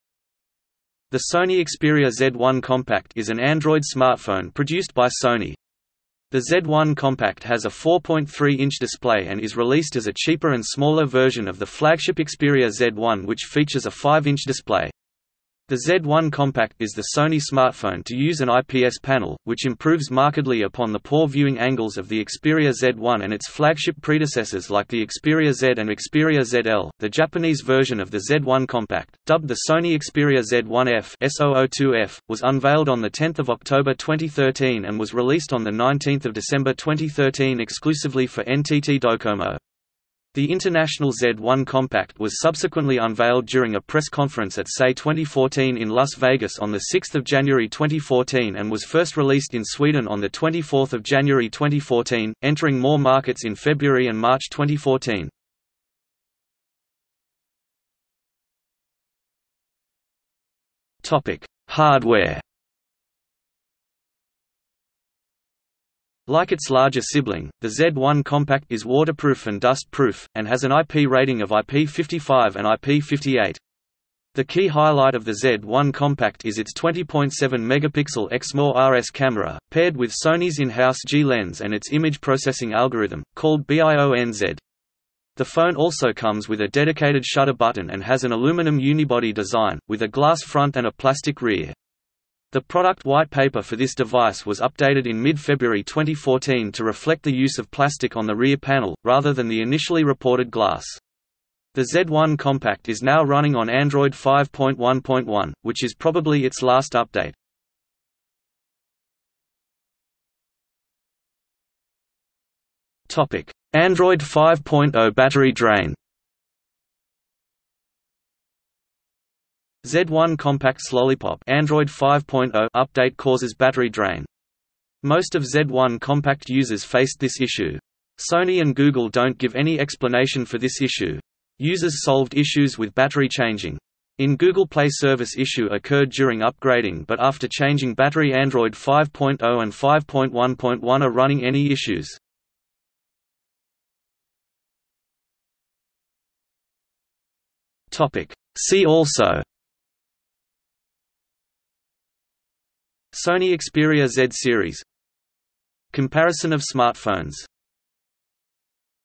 Sony Xperia Z1 Compact | Wikipedia audio article
00:00:57 1 Hardware
00:01:49 2 Android 5.0 battery drain
00:02:24 3 See also

- Socrates

SUMMARY
The Sony Xperia Z1 Compact is an Android smartphone produced by Sony. The Z1 Compact has a 4.3 inch display and is released as a cheaper and smaller version of the flagship Xperia Z1 which features a 5-inch display. The Z1 Compact is the Sony smartphone to use an IPS panel, which improves markedly upon the poor viewing angles of the Xperia Z1 and its flagship predecessors like the Xperia Z and Xperia ZL.The Japanese version of the Z1 Compact, dubbed the Sony Xperia Z1 f (SO-02F), was unveiled on 10 October 2013 and was released on 19 December 2013 exclusively for NTT DoCoMo. The international Z1 Compact was subsequently unveiled during a press conference at CES 2014 in Las Vegas on 6 January 2014 and was first released in Sweden on 24 January 2014; entering more markets in February and March 2014.